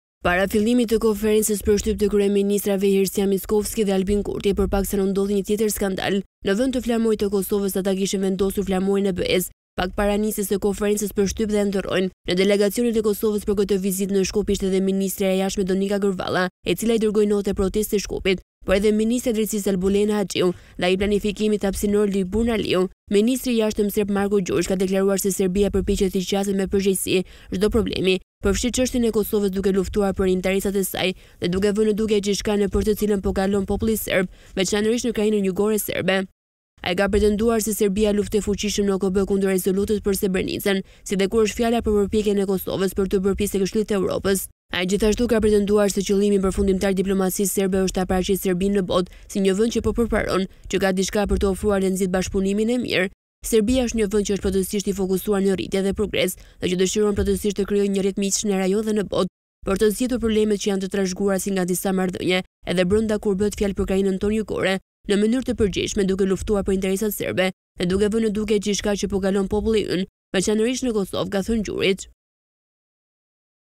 PAMJE VIRALE, ndodh edhe kjo në Shkup/ Kosovës i ngatërrohet flamuri
Sot para fillimit të konferencës për shtyp të kryeministrave Hristijan Mickoski dhe Albin Kurti për pak sa nuk ndodhi një skandal tjetër.
Në vend të flamurit të Kosovës e kishin vendosur flamurin e BE-së, pak para nisjes së konferencës për shtyp, e ndërrojnë.
Në delegacionin e Kosovës për këtë vizitë në Shkup ishte edhe ministrja e Jashtme, Donika Gërvalla, e cila i dërgoi notë proteste Shkupit, por edhe ministrja e Drejtësisë, Albulena Haxhiu, dhe ai i Planifikimit Hapësinor, Liburn Aliu.

𝐀𝐮𝐭𝐨𝐫 : NTV